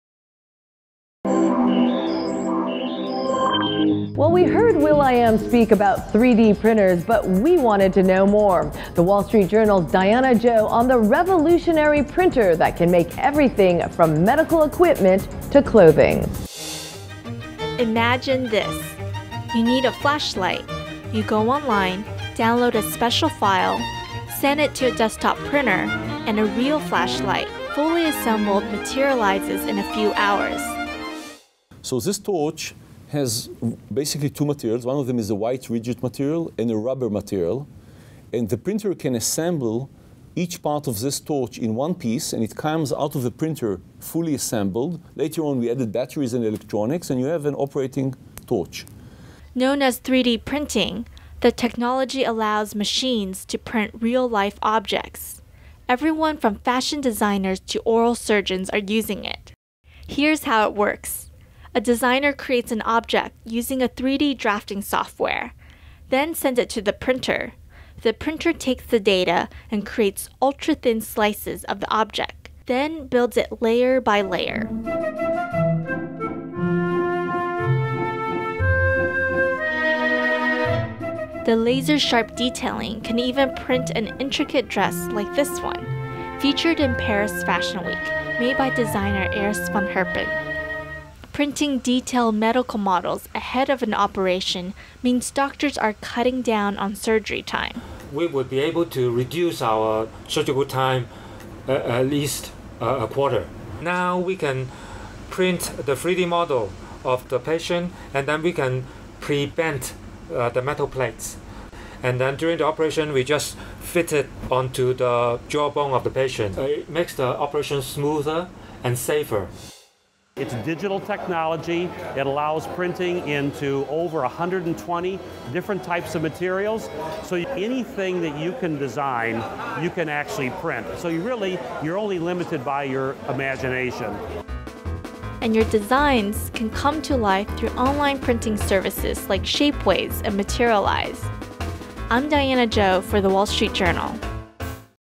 3D Printing: Imagination Is the Only Limit
3-D printing is a revolutionary new technology unlocking a world of innovation in design. The WSJ's Diana Jou explores everything from high fashion dresses to medical models made from nothing but a printer.